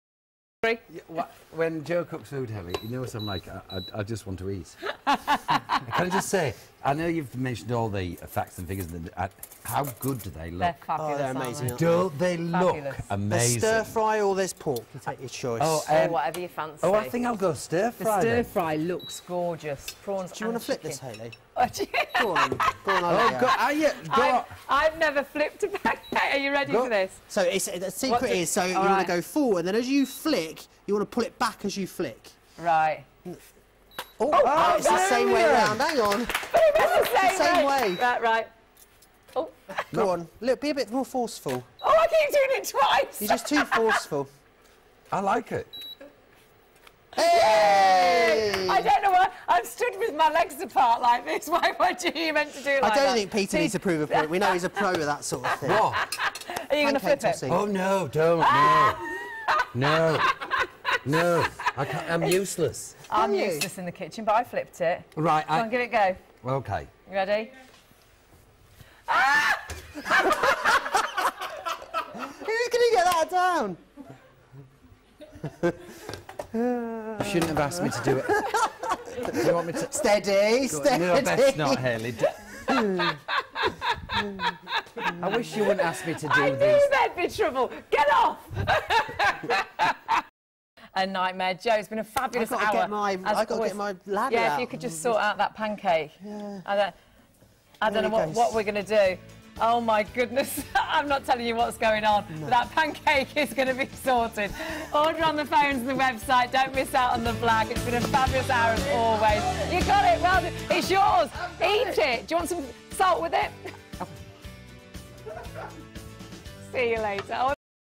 Ideal World - Peter Simon flips a pancake, funny
Peter Simon joins Hayley Green and Joe Remblance whilst selling pans.  Peter decides to try and flip a pancake with funny results!  Enjoy!